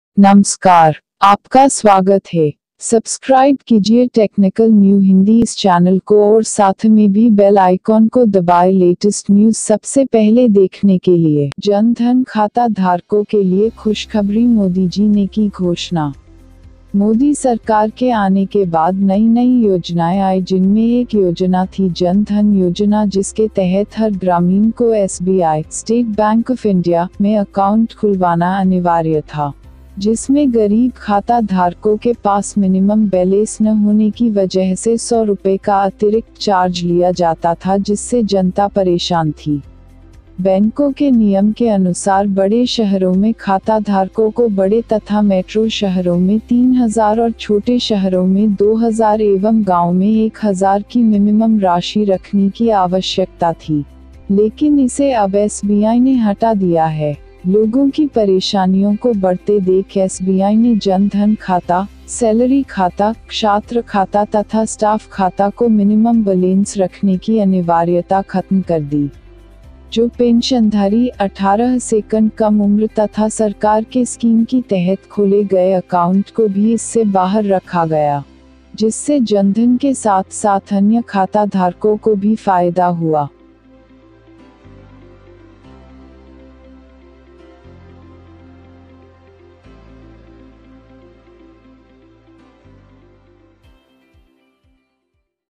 जन धन खाता धारको के लिये खुशखबरी मोदीजी ने की घोषणा.
मोदी सरकार के आने के बाद नई नई योजनाये आई जिनमे एक योजना थी जन धन योजना जिसके तहत हर ग्रामीण को SBI (State Bank Of India) में अकाउंट खुलवाना अनिवार्य था। जिसमे गरीब खाता धारको के पास मिनिमम बैलेंस न होने की वजह से 100 Rs का अतिरिक्त चार्ज लिया जाता था जिससे जनता परेशान थी।

बैंको के नियम के अनुसार बड़े शहरो में खाताधारको को बड़े तथा मेट्रो शहरो में 3000 और छोटे शहरो में 2000 एवं गाँव में 1000 की मिमिमम राशि रखनी की आवश्यकता थी।लेकिन इसे अब SBI ने हटा दिया है, लोगों की परेशानियों को बढ़ते देख SBI ने जन धन खाता, सैलरी खाता, क्षात्र खाता तथा स्टाफ खाता को मिनिमम बलेंस रखने की अनिवार्यता ख़त्म कर दी।

जो पेंशनधारी, 18 से कम उम्र तथा सरकार के स्कीम की तहत खोले गए अकाउंट को भी इससे बाहर रखा गया। जिससे जनधन के साथ साथ अन्य खाताधारको को भी फायदा हुआ।

Best Music Player App-

Download, Install & Play...